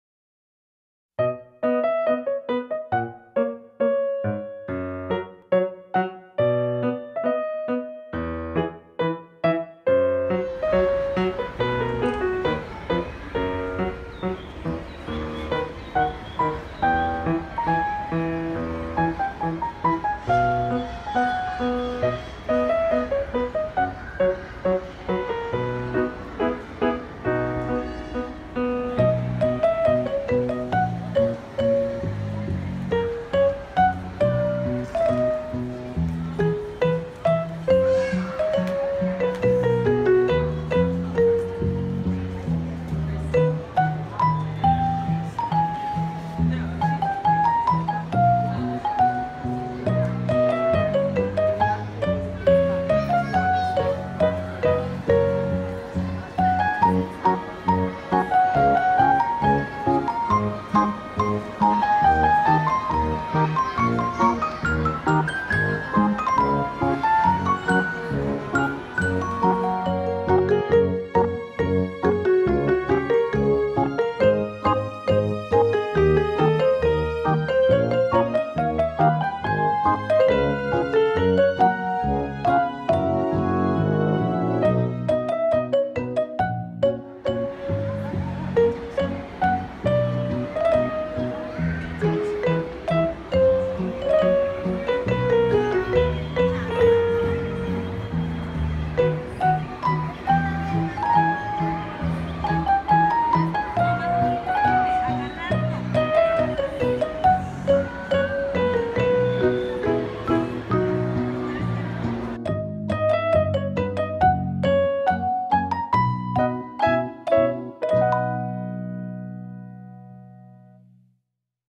SPRING HOPS INTO SOUTH COAST PLAZA
SPRING HAS ARRIVED at South Coast Plaza Shopping Center, Costa Mesa, CA.  This world famous shopping destination has one of the best Spring decorations anywhere.  Bring the kids to see the Spring Garden and the Easter Bunny.